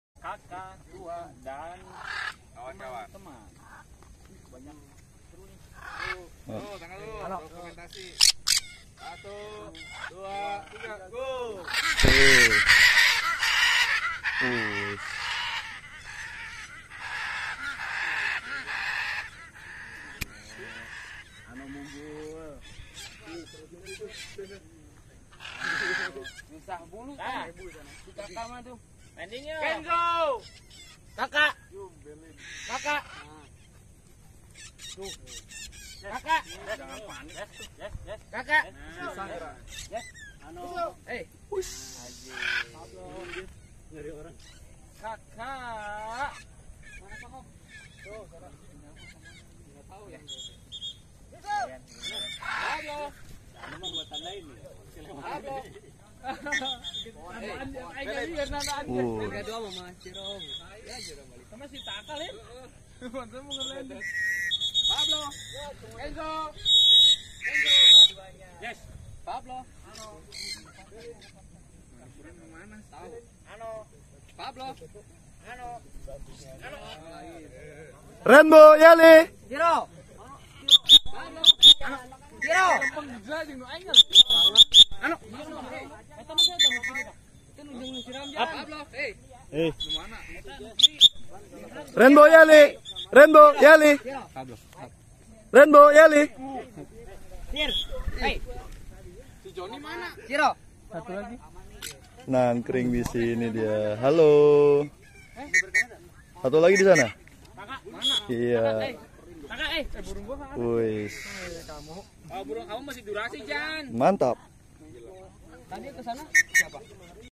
Sun Conure Free Fly Bareng Macaw, Betet, Bayan, dan Kakak Tua
sunconure freefly dengan macaw, kakaktua, betet jawa, dan bayan 17 April 2022 di Bogor.